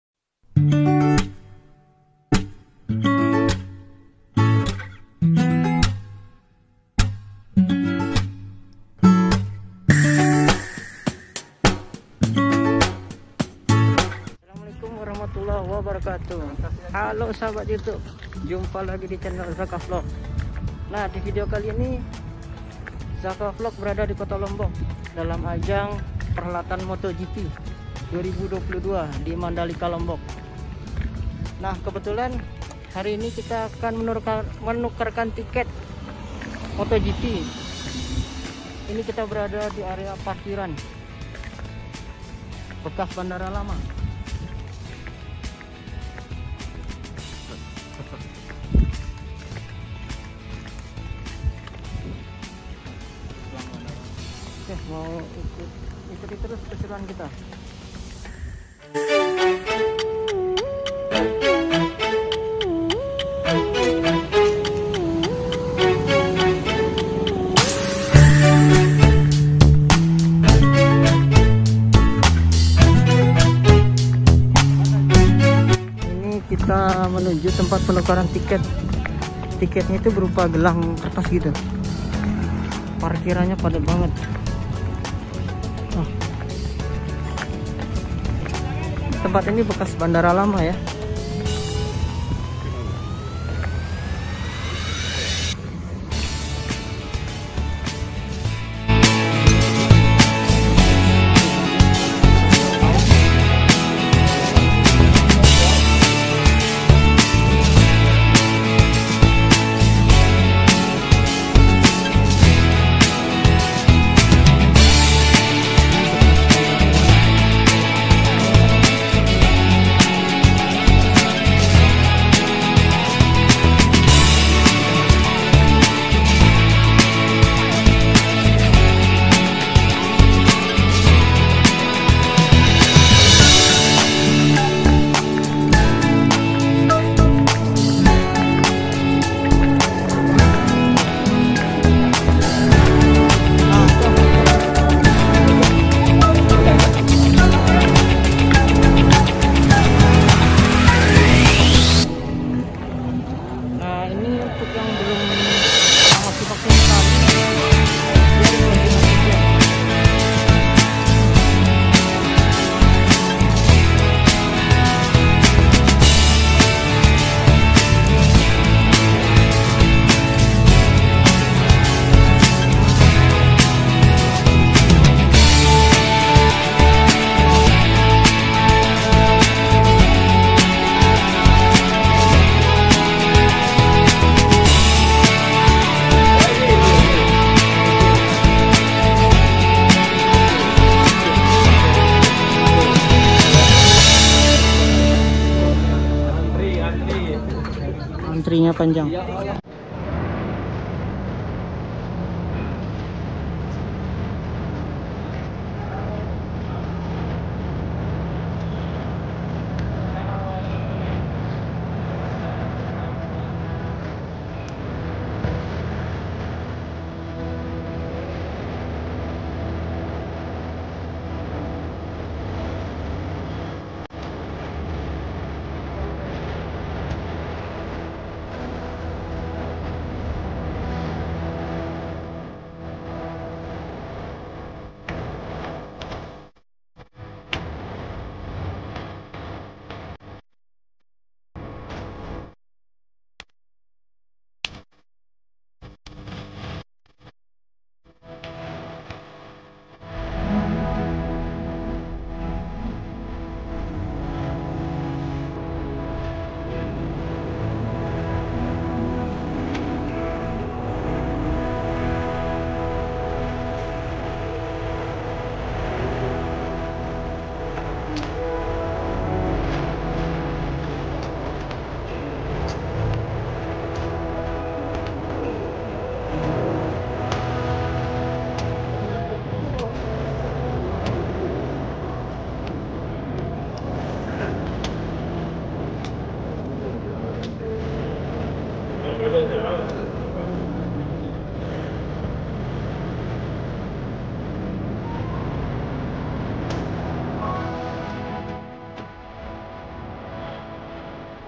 Penukaran Tiket Moto Gp Mandalika 2022 || Sampai 3 Jam Antri
Penukaran tiket motoGp Mandalika 2022,yg di adakan di ex bandara selaparan.sunggu sangat ramai sekali,,sampai ada yg mengantri 3 jam,hanya untuk mendapatkan gelang,untuk menonton motogp